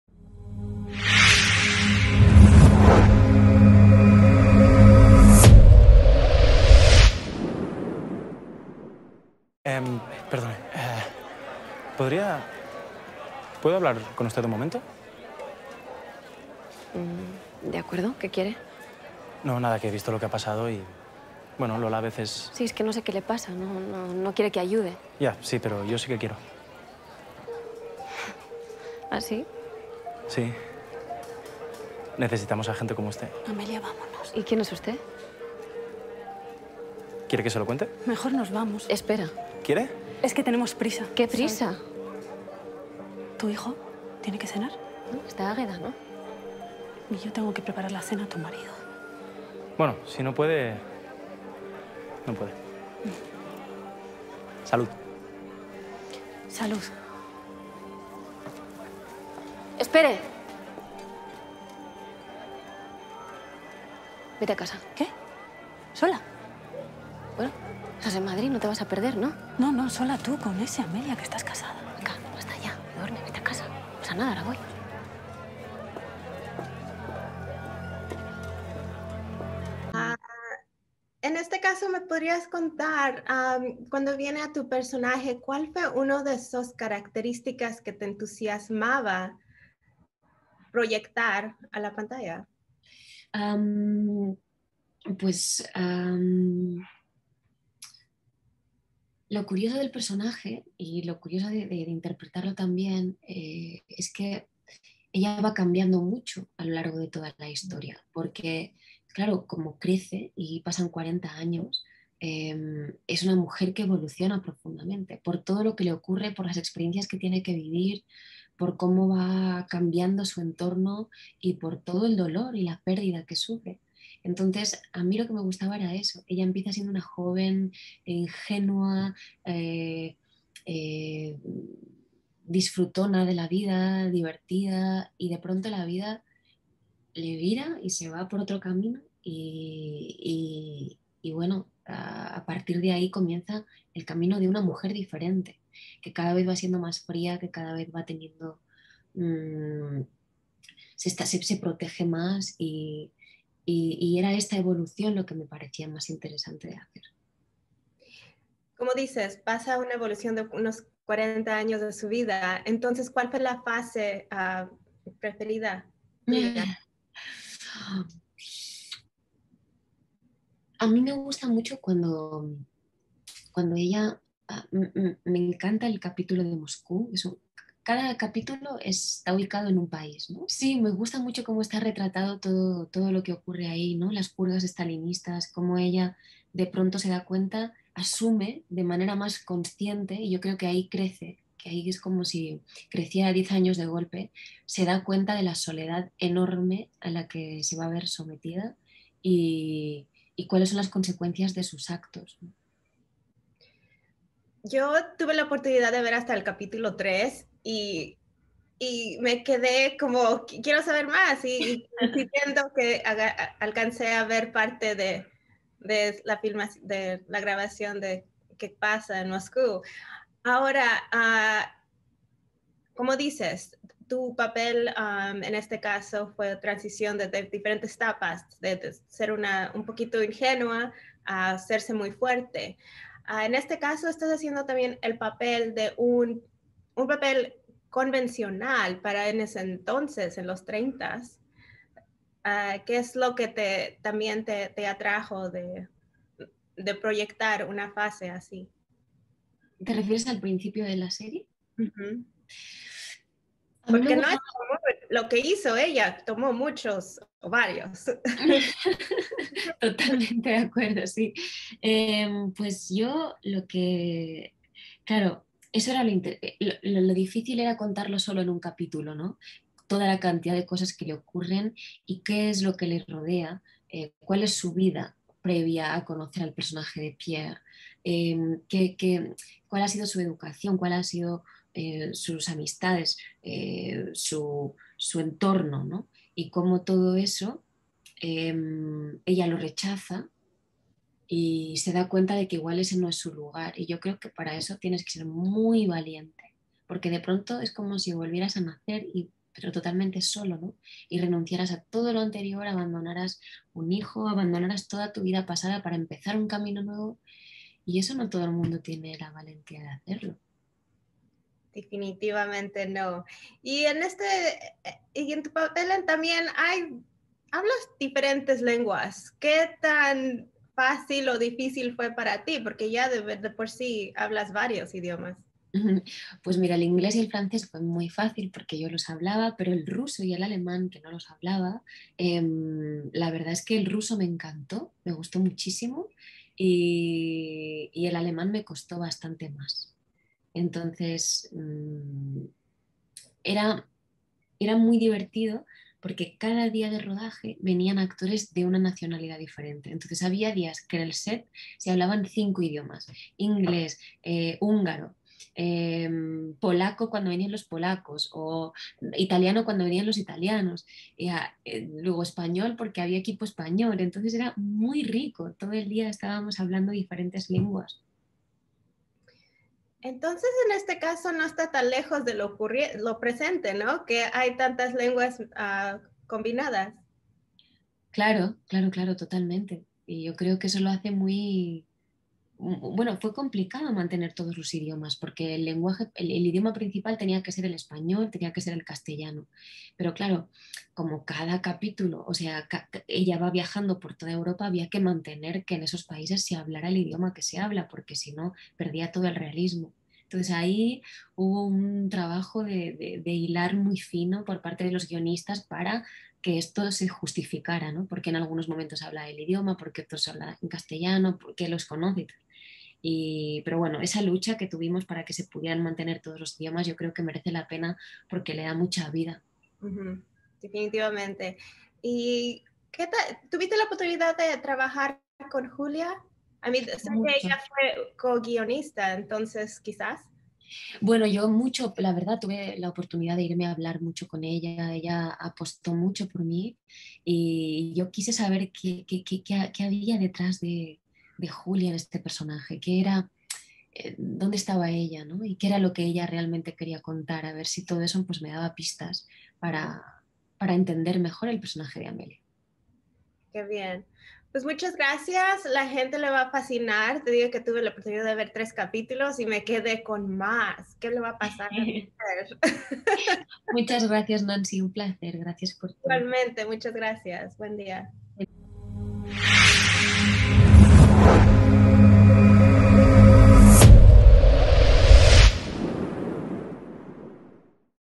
Irene Escolar Habla De Su Papel Estelar en Dime Quién Soy: Mistress of War de Peacock
Nancy Tapia de LRMOnline entrevistó a la Irene Escolar para la nueva serie Dime Quién Soy: Mistress of War en Peacock

Basada en la novela "Dime quién soy" de Julia Navarro, que alcanzó récord de ventas, DIME QUIÉN SOY: MISTRESS OF WAR cuenta la historia de la resiliencia Amelia Garayoa (Irene Escolar), una socialité madrileña que repentinamente se ve sumergida en el mundo del espionaje internacional.

Los nueve episodios de Dime Quién Soy : Mistress of War ya están disponibles por Peacock

DIME QUIÉN SOY: MISTRESS OF WAR es una producción de Movistar+ y Telemundo International Studios en colaboración con DLO Producciones. Creada por José Manuel Lorenzo, dirigida por Eduard Cortés y escrita por Piti Español, la serie cuenta con la producción ejecutiva de Domingo Corral (Movistar+), José Manuel Lorenzo (DLO Producciones) y Marcos Santana (Telemundo International Studios).

Entrevistadora: Nancy Tapia (@inancytapia on IG)